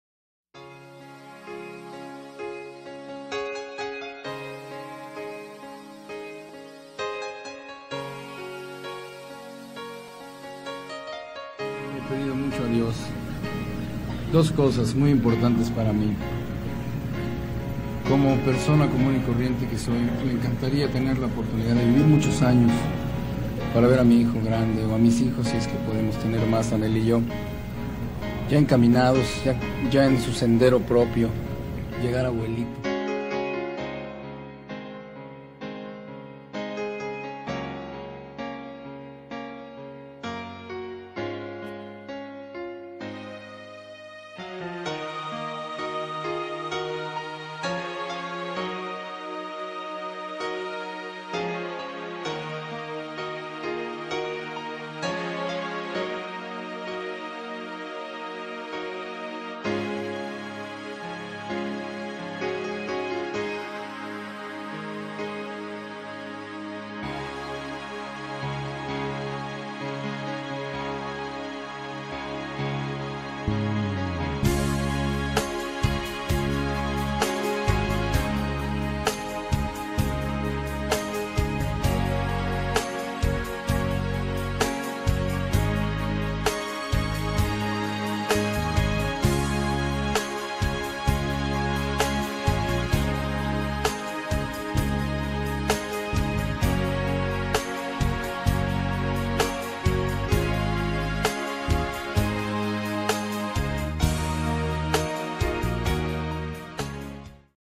Momentos únicos e imborrables de Anel, José Sosa y sus hijos
Momentos únicos e imborrables vividos por la familia Sosa Noreña, plasmados en hermosas fotografías. José durante una entrevista en el año 1980, expresó  acerca  de que una de las peticiones que el hacía a Dios era tener muchos años de vida para ver grande a su hijo Pepito y a los hijos que pudiera tener con Anel. Escúchalo en su propia voz en ésta historia!